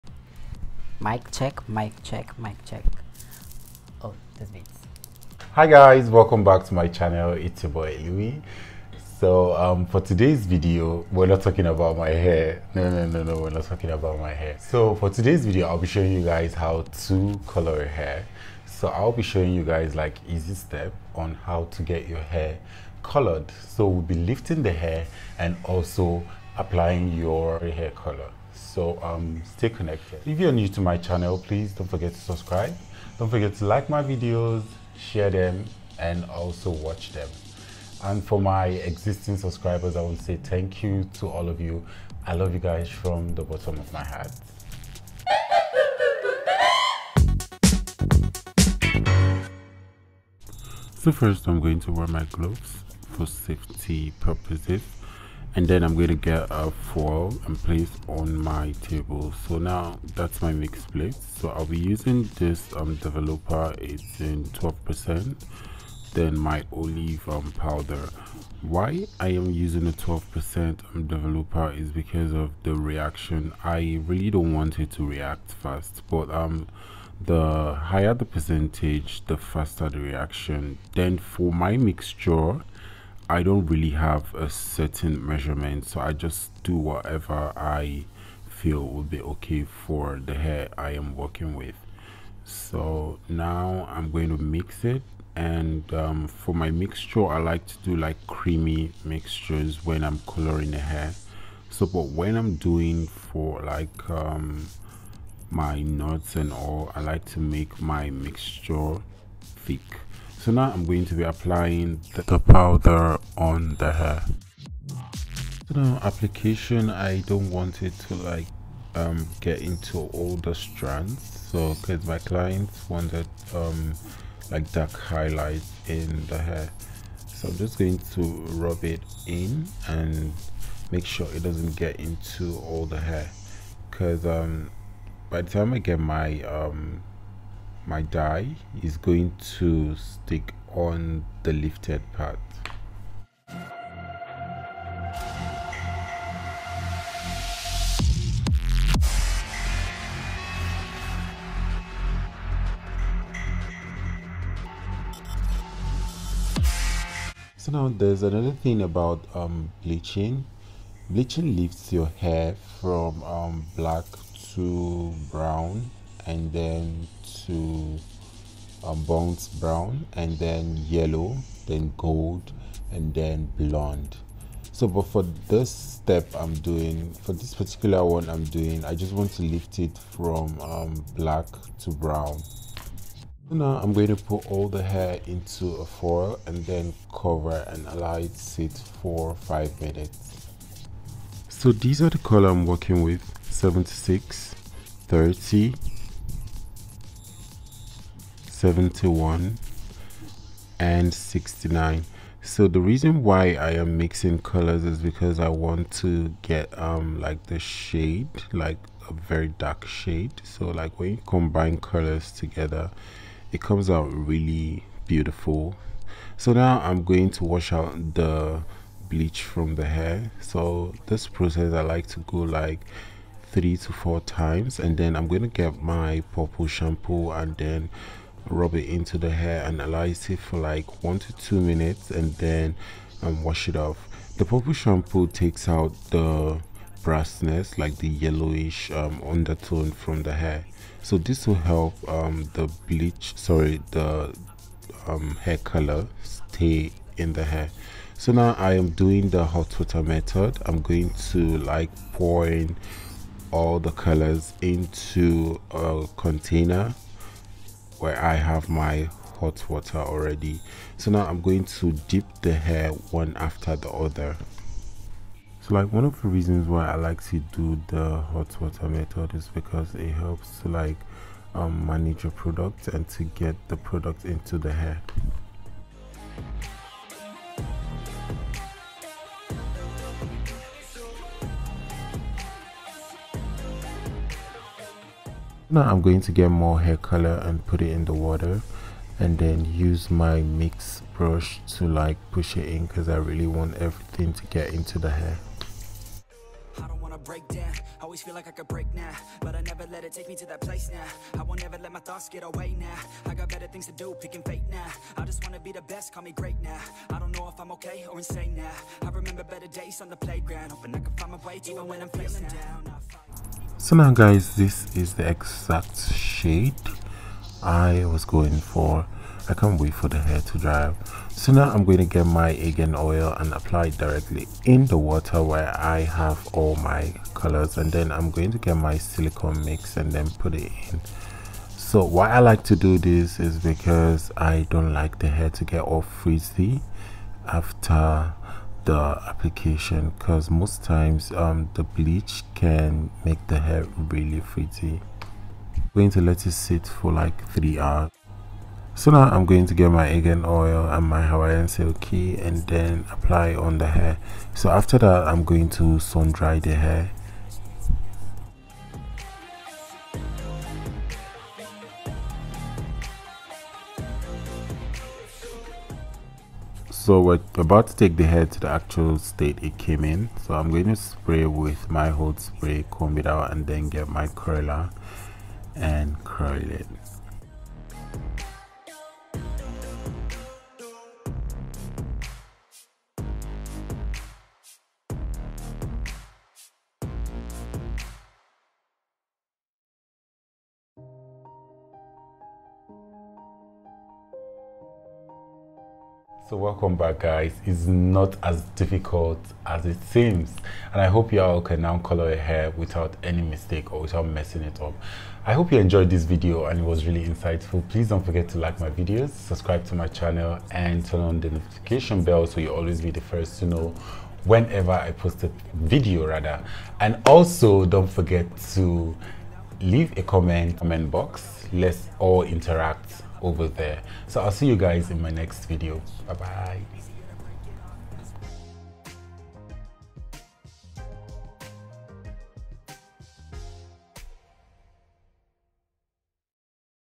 HOW TO LIFT AND COLOR A HAIR USING BLEACH, DEVELOPER, & ADORE DYE
Hey guys
So in this video I’ll be showing how to color a hair. From lifting to color application.

Thanks